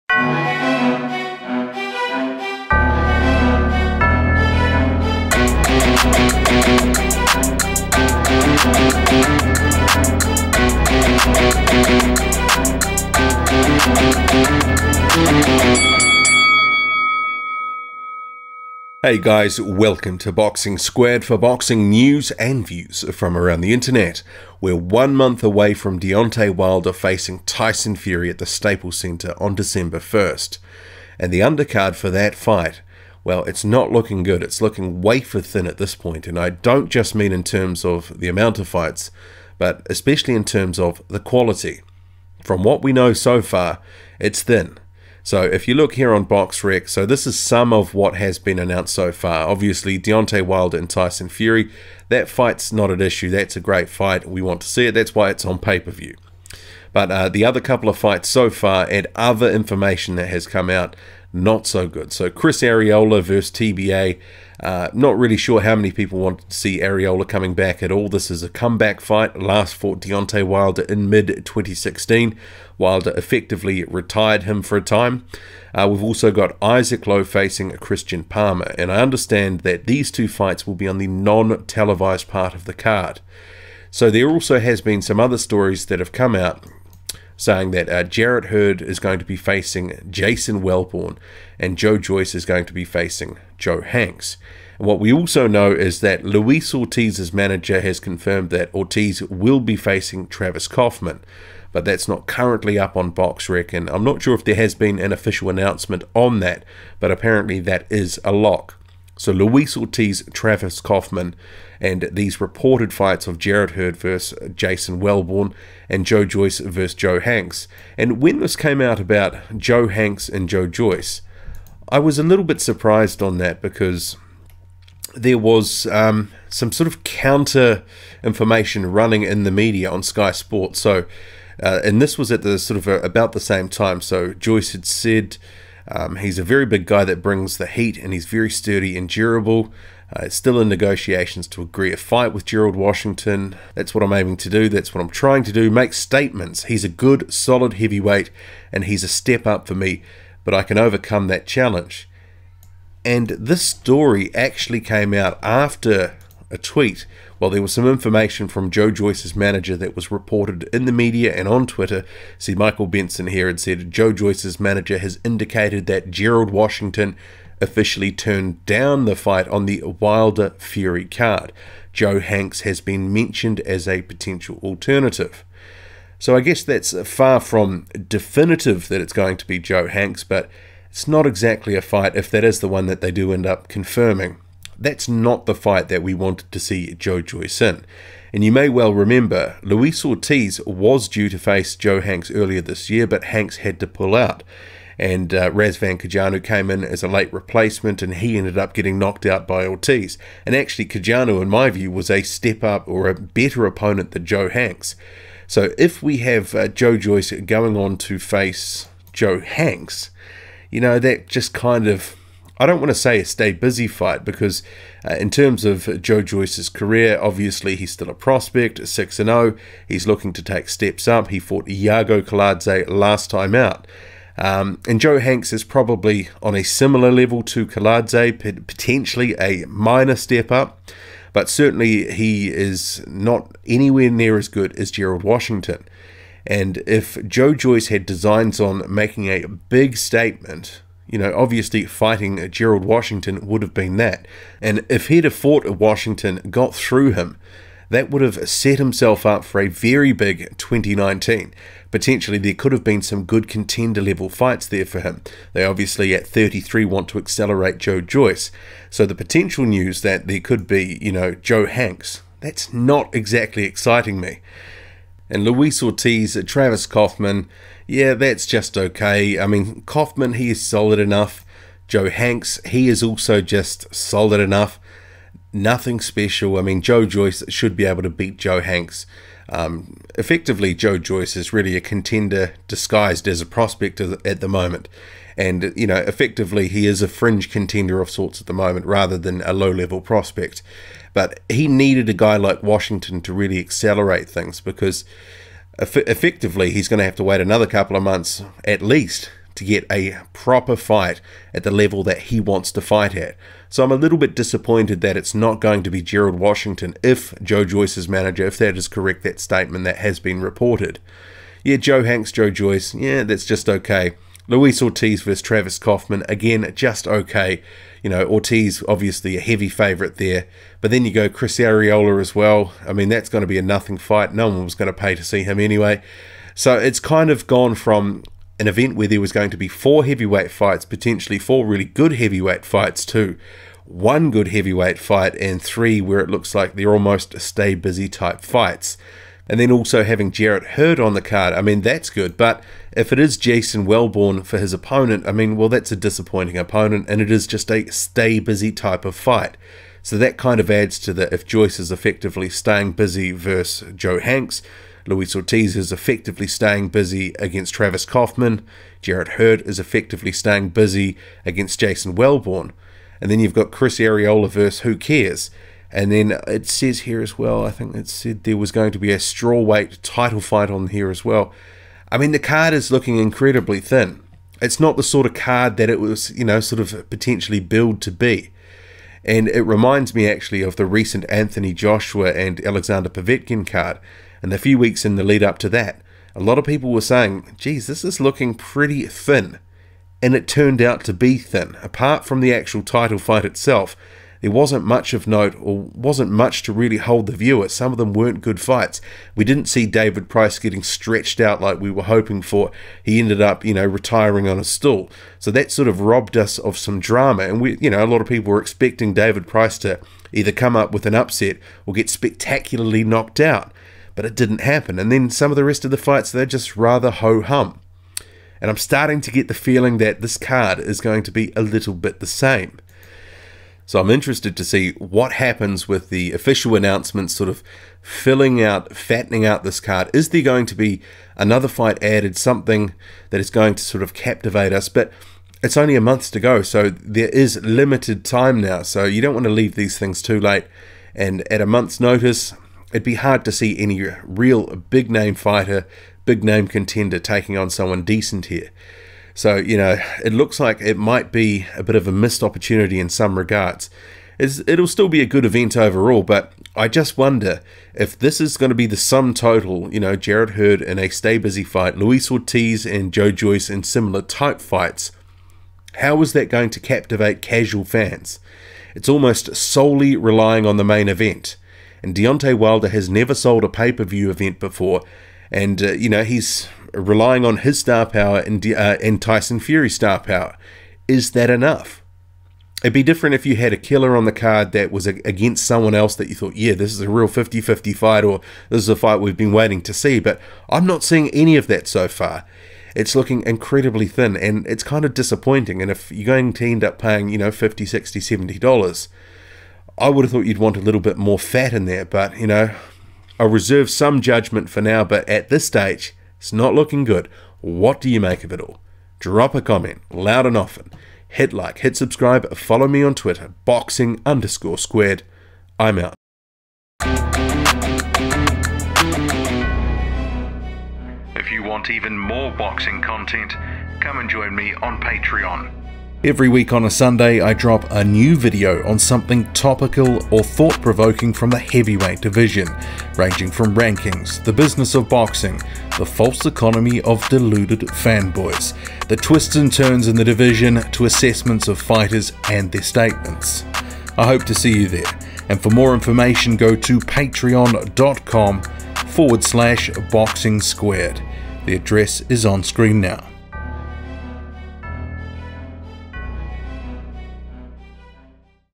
DEONTAY WILDER & TYSON FURY UNDERCARD LOOKING THIN
The undercard for the Deontay Wilder & Tyson Fury PPV event on December 1 is looking very thin.

Boxing Squared